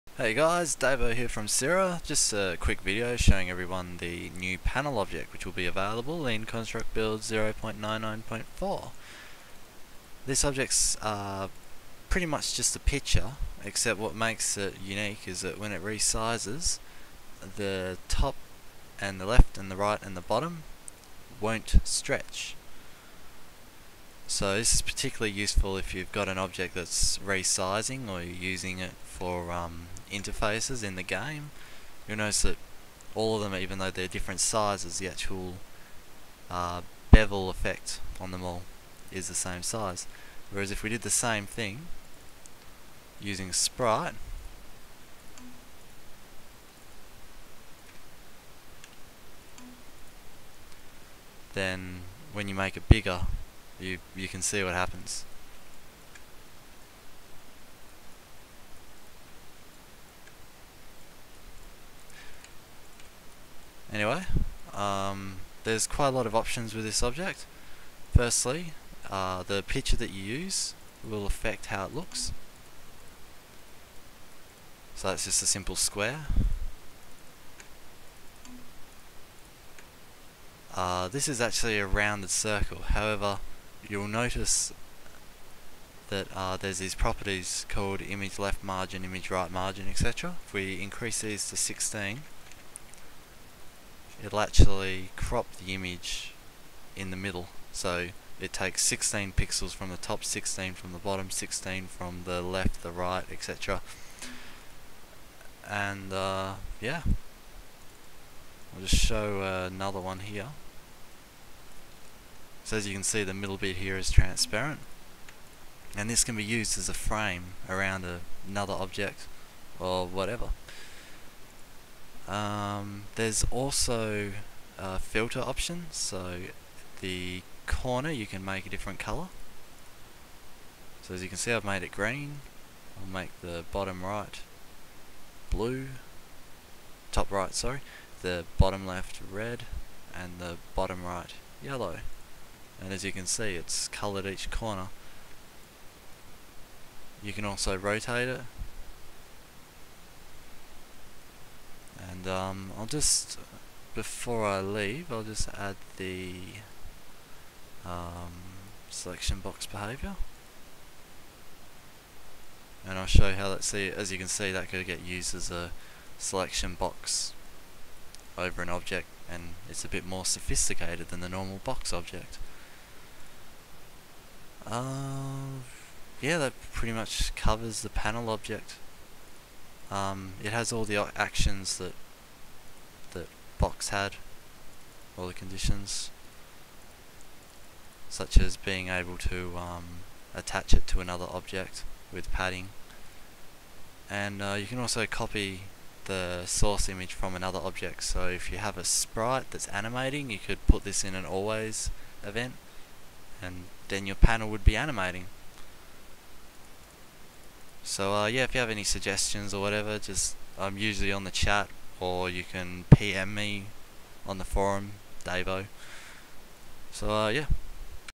Panel Object
This is a new plugin that will be available in Construct 0.99.4

Very handy for situations where you want a box which wont look 'stretched'